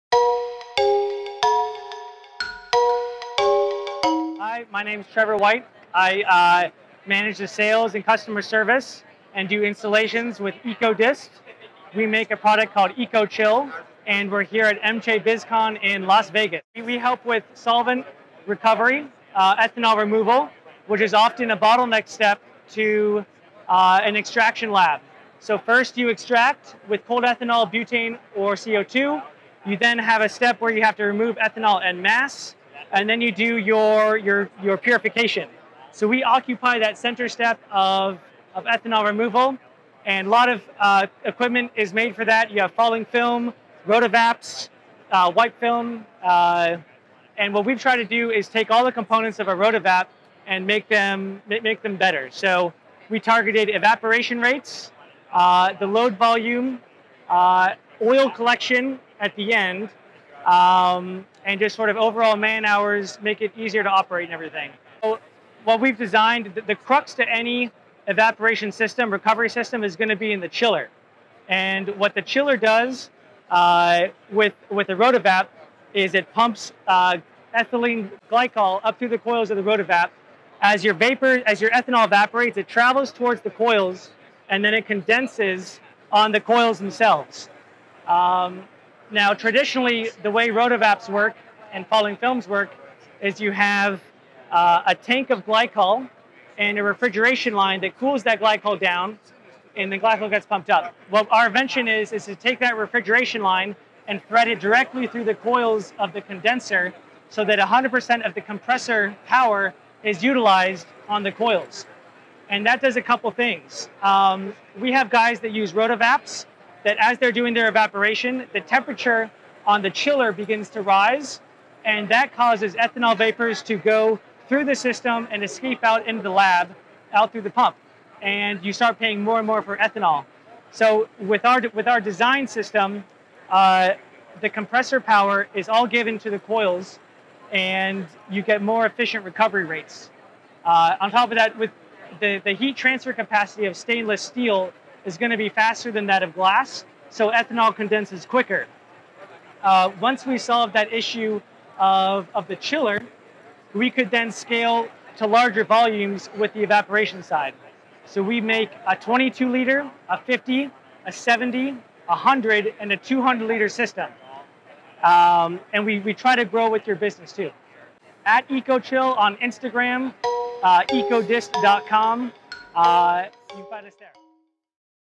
The Ecochyll X7 by Ecodyst - Solvent Recovery System
We caught up with Trevor White, Installer and Customer Service Manager of Ecodyst, at the MJBizCon 2018 in Las Vegas. He showed us the problems Solvent Recovery Systems are facing and what Ecodyst to solve those problems!

Why is the EcoChyll X7 efficient, innovative, and eco-friendly?

Also make sure to follow our other social media platforms to stay up-to-date on all things Cannabis & Tech Today.

Video: Adam Saldaña